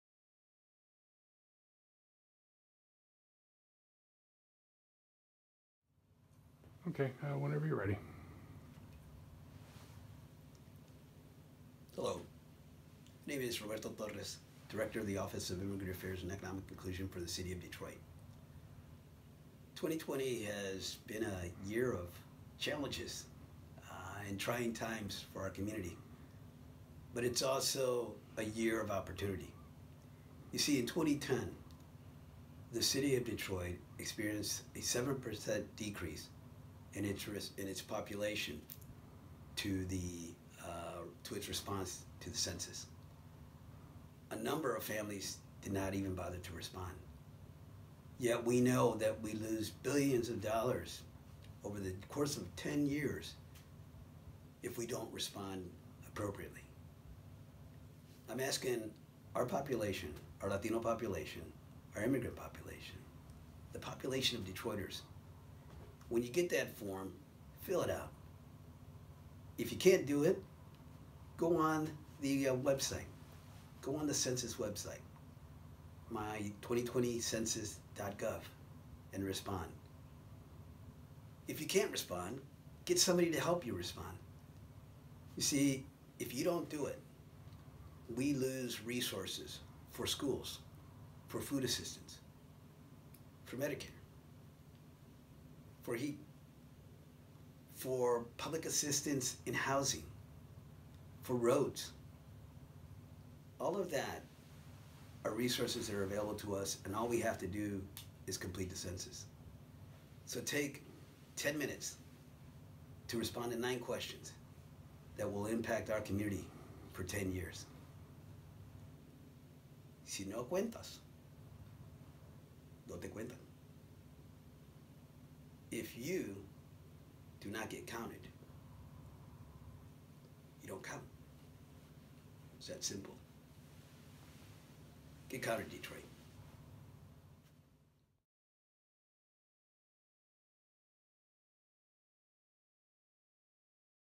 Roberto Torres on the 2020 Census
Roberto Torres the Director of Immigrant Affairs & Economic Inclusion for the City of Detroit explains why 2020 Census Participation is vitally important.